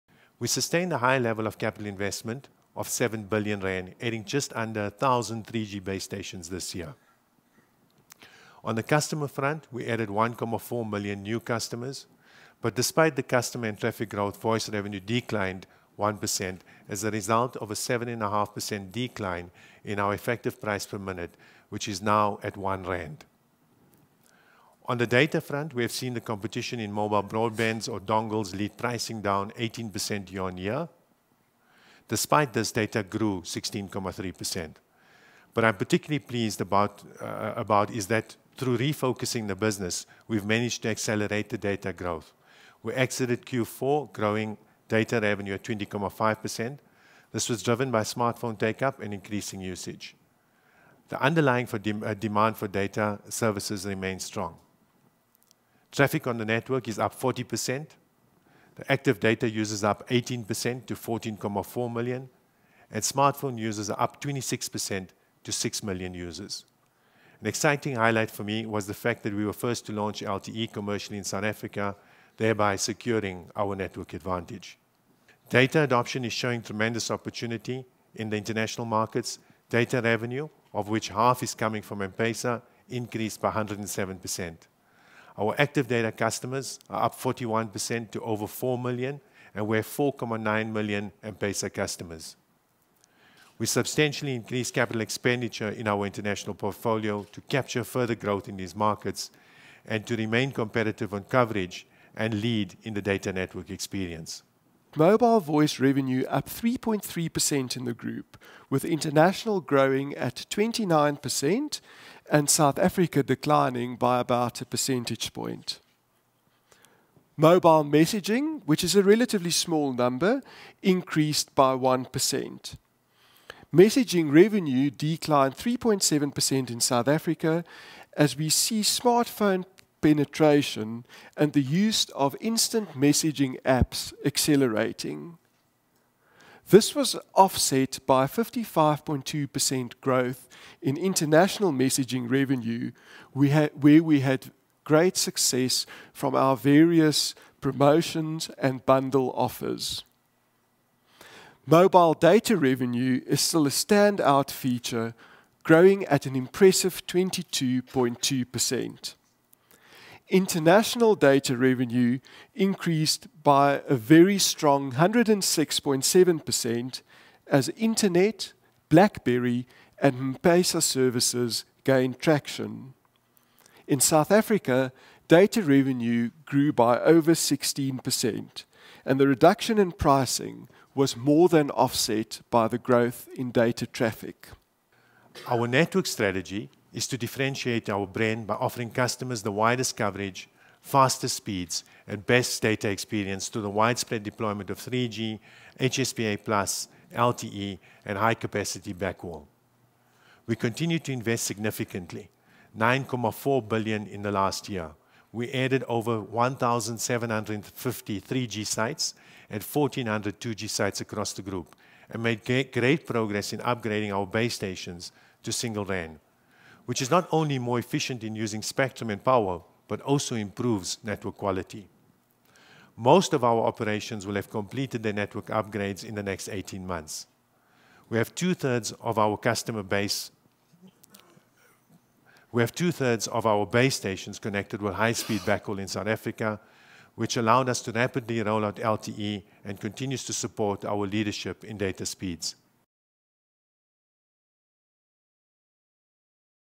Data to make up 30% of Vodacom's revenue in 3 years
As data adoption accelerated, JSE-listed telecommunications group Vodacom aimed to secure 30% of its revenue from data traffic within the next three years, CEO Shameel Joosub said on Monday.